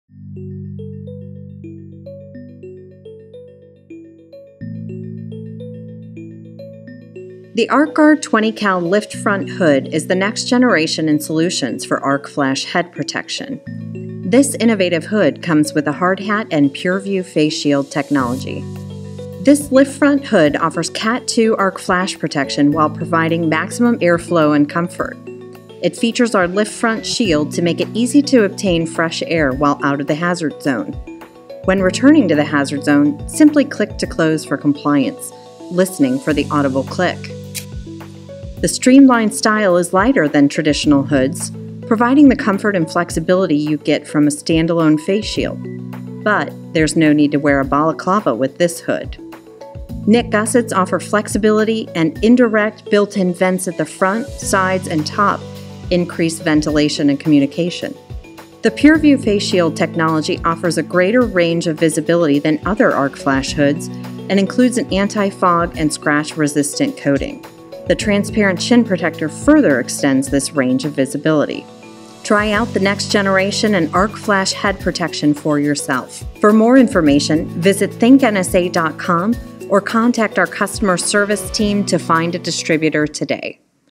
ArcGuard 20 Cal Lift Front Arc Flash Hood
Product overview for NSA's ArcGaurd 20 Cal Lift Front Arc Flash Hood with PureView faceshield technology.